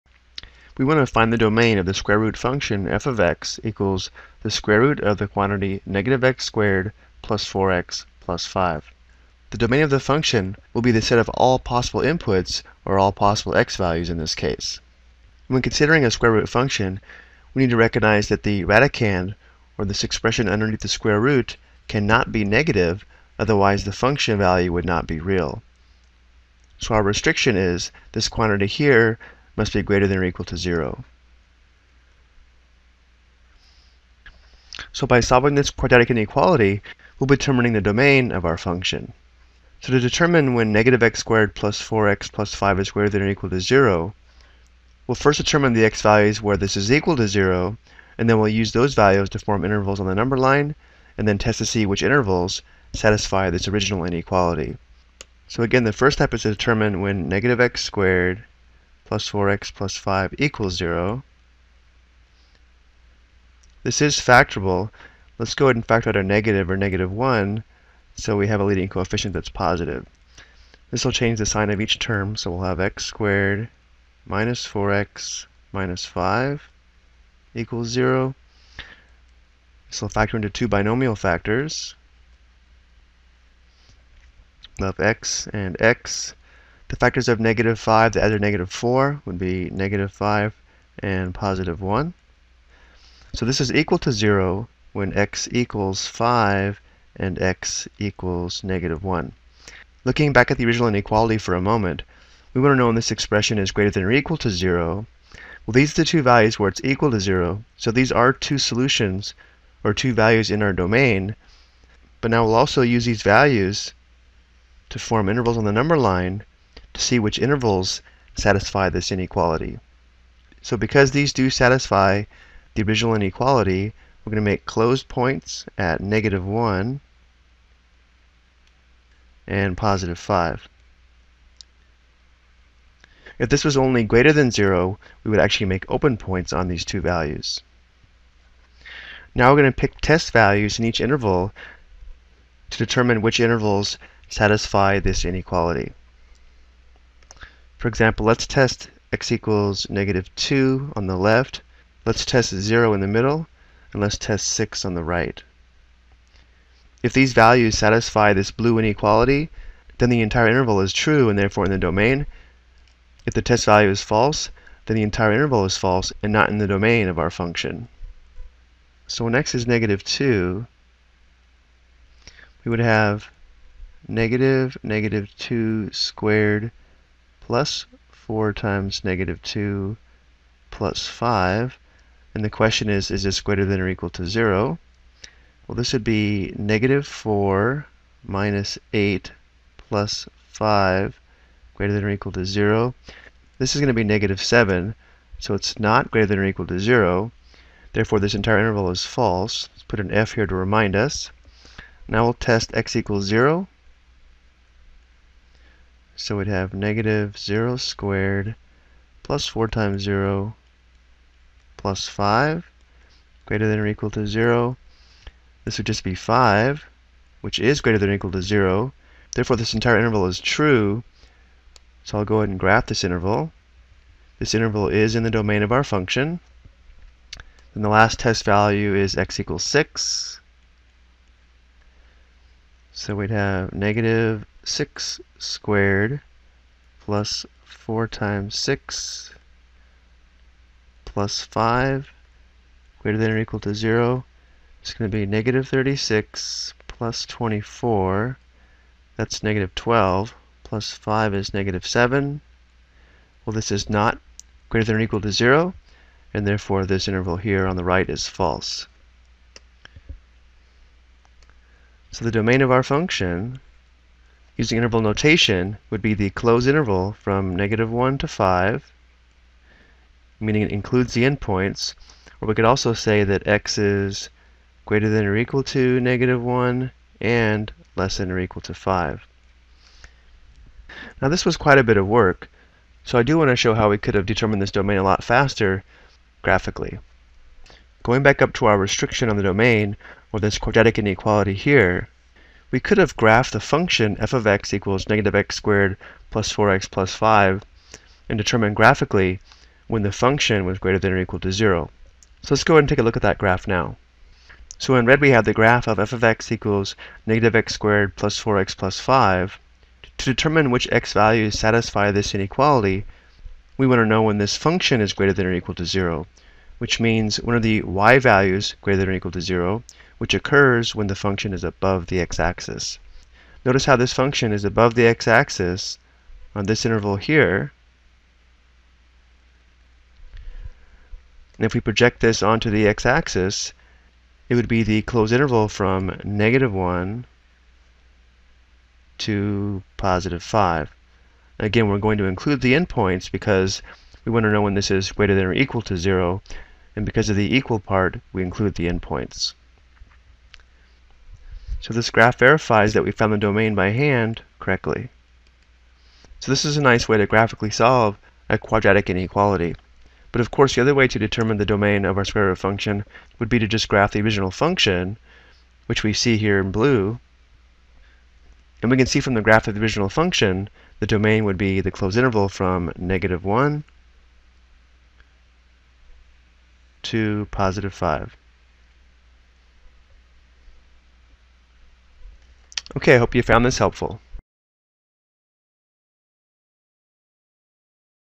Ex: Domain of a Square Root Function with a Quadratic Radicand
This video explains several ways to find the domain of a square root function when the radicand is a quadratic function.